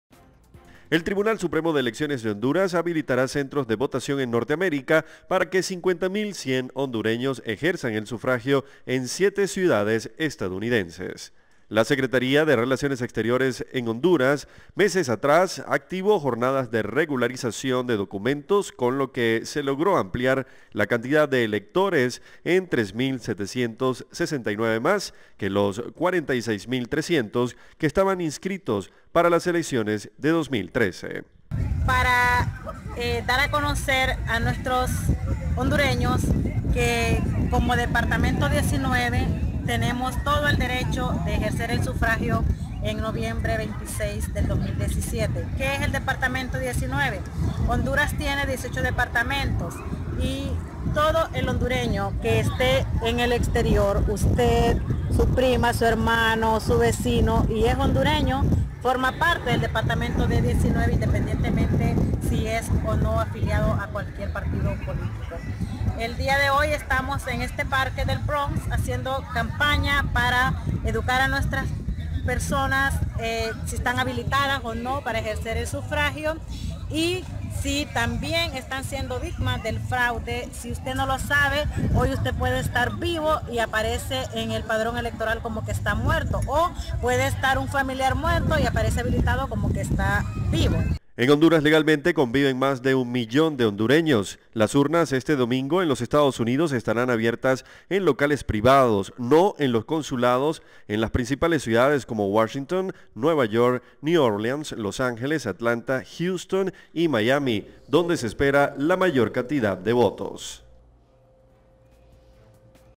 ¿Cómo votarán los hondureños en el exterior este 26 de noviembre?
Este domingo, 26 de noviembre, los hondureños acudirán a las urnas para elegir al próximo presidente.

Más de 50.000 hondureños votarán en Estados Unidos. Los centros de votación se dispondrán en locales privados, fuera de los consulados, en las ciudades de Washington, Nueva York, New Orleans, Los Ángeles, Atlanta, Houston y Miami.